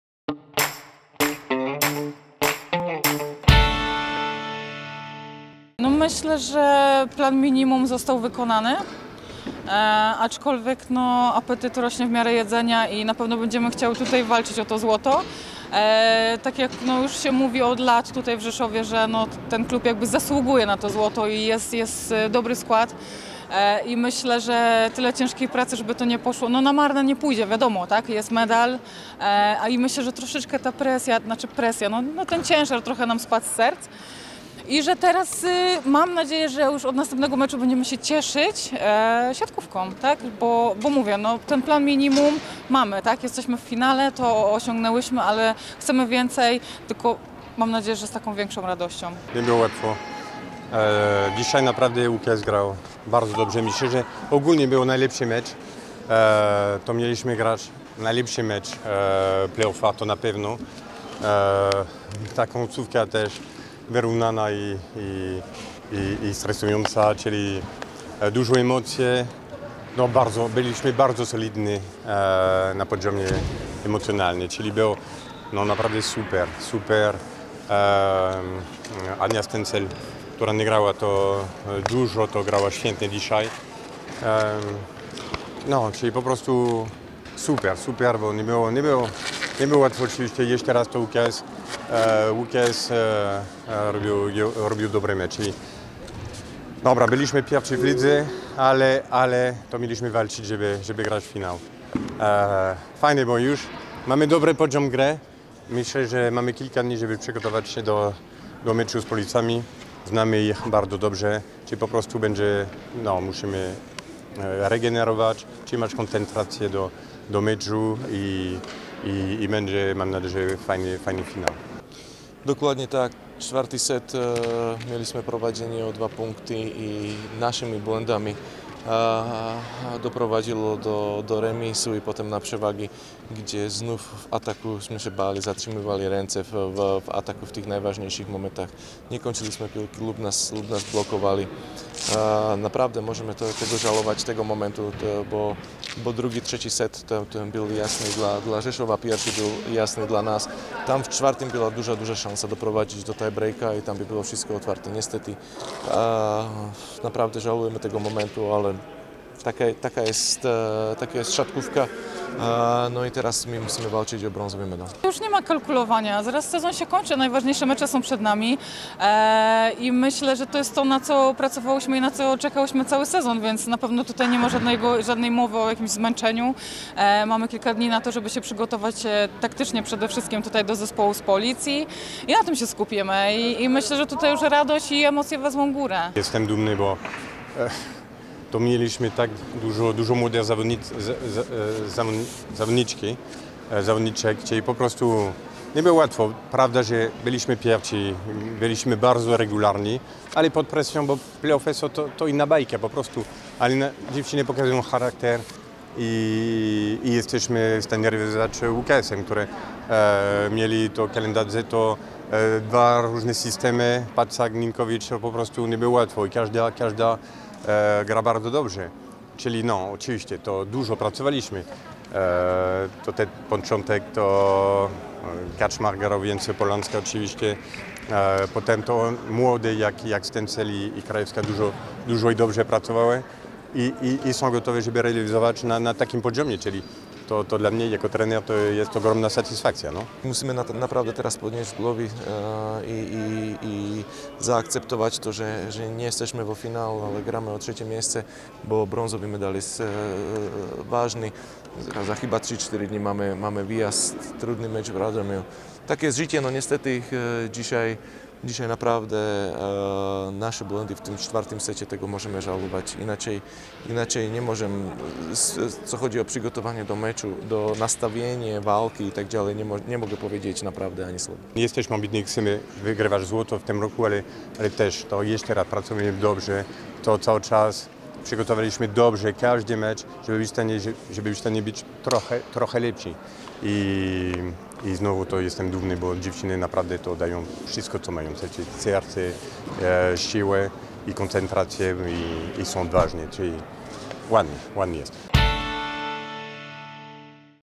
Pierwszy historyczny finał Developresu
Developres SkyRes Rzeszów po raz drugi pokonał łodzianki i to zespół trenera Stephane’a Antigi zagra w zagra w wielkim finale TAURON Ligi. Zmierzy się w nim z Grupą Azoty Chemikiem Police.